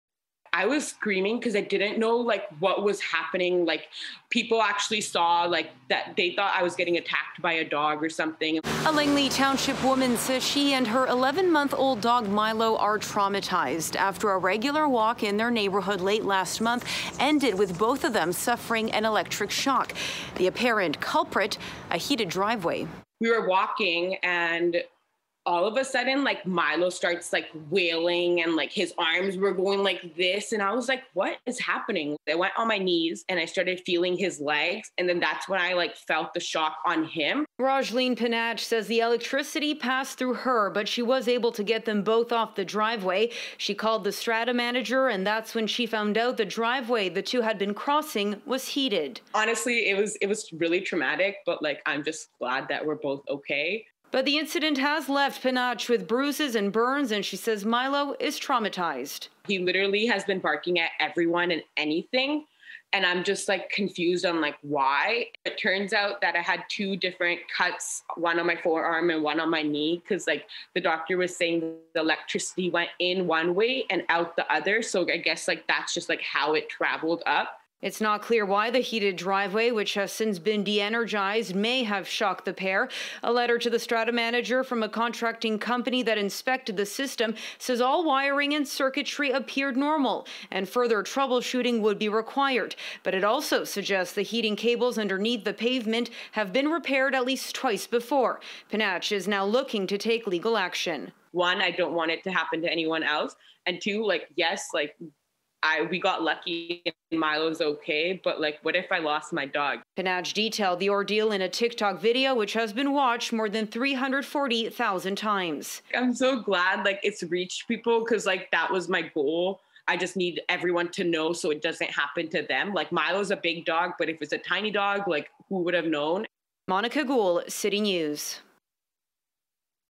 Langley, B.C. woman, dog traumatized after suffering electric shock from heated driveway
EXCLSUIVE: A regular walk in the neighborhood has left a woman and her dog in the Township of Langley with physical and emotional scars. Monika Gul has the story.